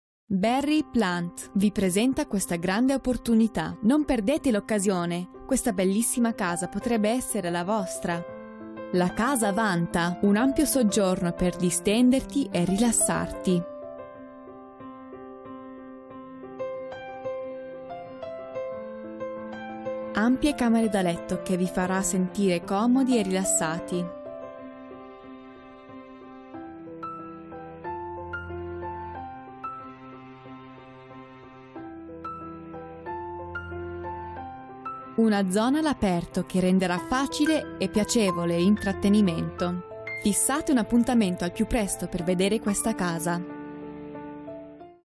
For Sale 2 Eaglefarm Court, Keilor Park - Italian
For Sale -  2 Eaglefarm Court, Keilor Park Barry Plant Niddrie 3 Bedrooms 2 Bathrooms 1Garage   Elegant Family Entertainer Move in, enjoy and relax, this three bedroom, two bathroom family haven has been smartly renovated and combines a selection of superb spaces with quality features. Presenting a generous family/living, well designed kitchen with 900mm stove, adjoining dining domain and luxuriously large covered entertainer’s deck, you’ll love sharing this home with family and friends. Bedrooms host BIRs and two gleaming modern bathrooms include a concealed ensuite and main bathroom with freestanding tub. Top extras include heating/cooling, an automatic garage and two driveways, ensuring outstanding off street parking space. Situated in a small court a stroll to transport and Keilor Park Recreation Reserve, close to Fosters Road shops and a quick drive to schools including St Christopher’s Primary School and Penleigh & Essendon Grammar, this is a family friendly opportunity not to be missed.